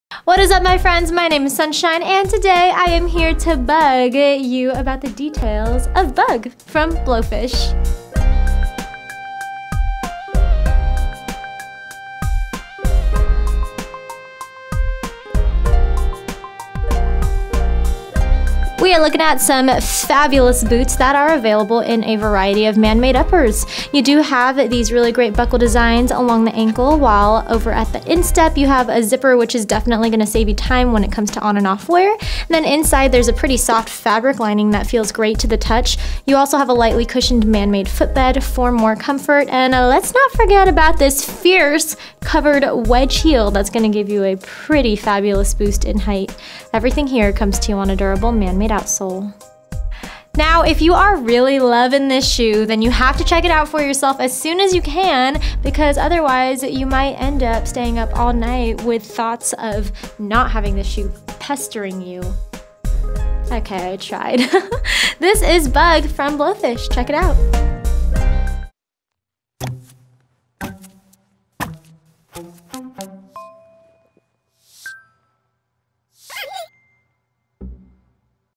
Blowfish Bug SKU:8719658
Get the lead out and set out for a great day in the Blowfish™ Bug boots!
Available in a variety of man-made uppers.
Decorative buckle straps with a hidden zip closure.
Fabric lining.
Lightly cushioned man-made footbed.
Covered wedge heel.
Man-made sole.
Imported.
Measurements:

    Heel Height: 2 1⁄4 in
    Weight: 10 oz
    Circumference: 9 3⁄4 in
    Shaft: 5 in